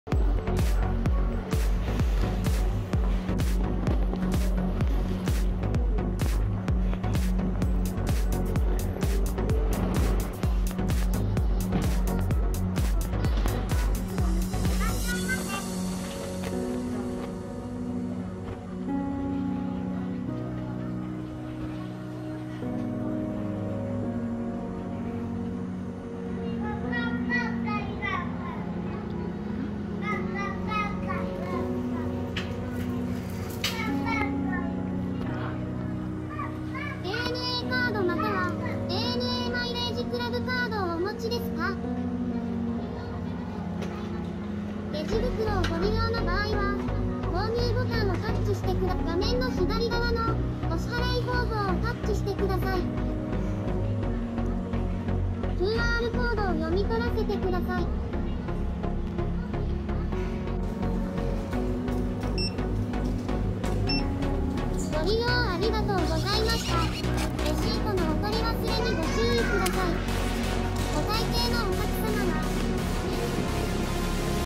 無人決済ギフト店「ANA FESTA GO羽田B1フロア店」へ約1年振り訪れる（2022年7月某日）
TOUCH TO GOを次に導入したのは国内初の空港ギフトショップ !!羽田空港第２ターミナルB１に無人決済システムを活用した「ANA FESTA GO」を8/27にオープン